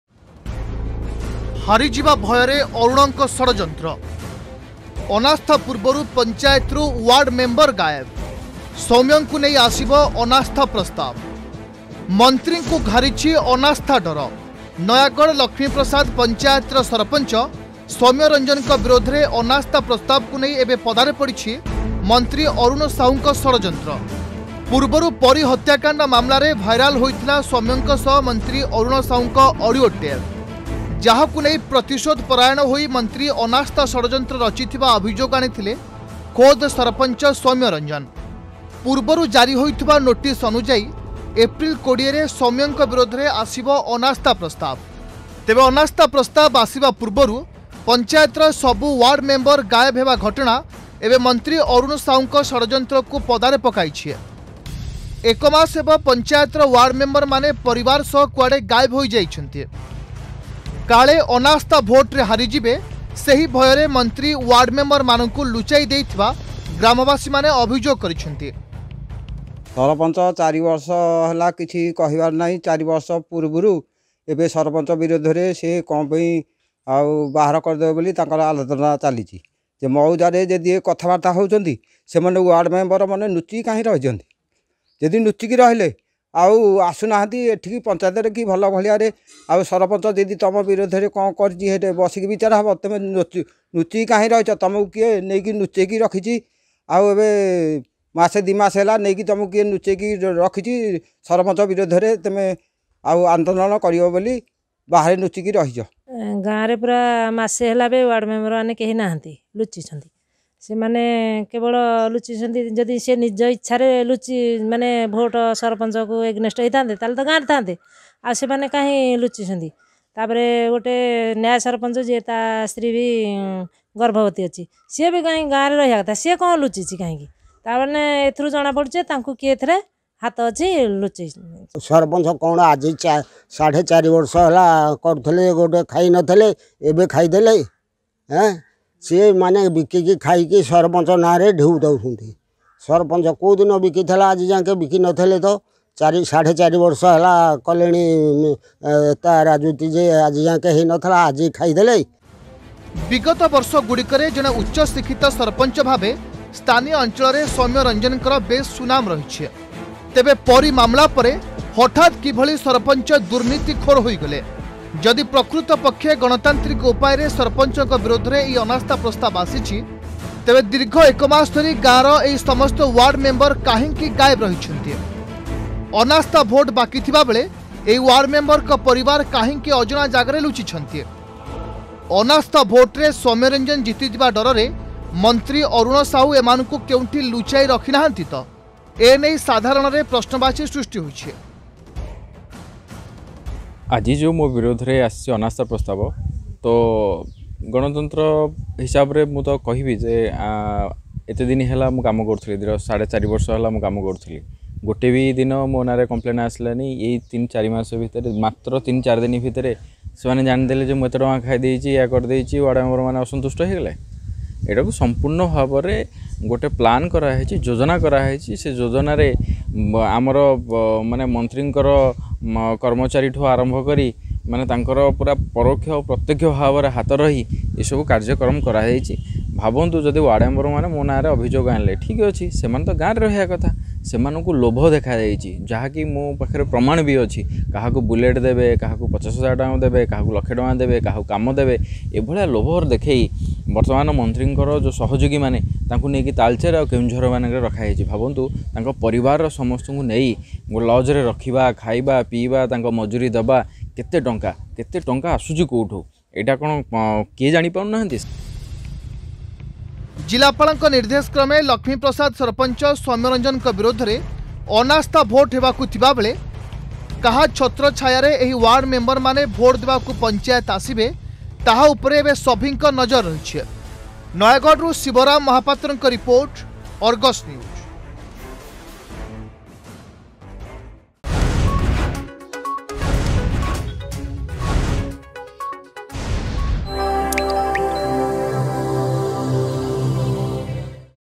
ହାରିଯିବା ଭୟରେ ଅରୁଣଙ୍କ ଷଡଯନ୍ତ୍ର
ସୌମ୍ୟଙ୍କୁ ନେଇ ଆସିବ ଅନାସ୍ଥା ପ୍ରସ୍ତାବ...ମନ୍ତ୍ରୀଙ୍କୁ ଘାରିଛି ଅନାସ୍ଥା ଡର...ଅନସ୍ଥା ଭୋଟରେ ହାରିଯିବା ଭୟରେ ମନ୍ତ୍ରୀ ଓ୍ବାର୍ଡମେମ୍ବର ମାନଙ୍କୁ ଲୁଚାଇଥିବା ଗ୍ରାମବାସୀଙ୍କ ଅଭିଯୋଗ...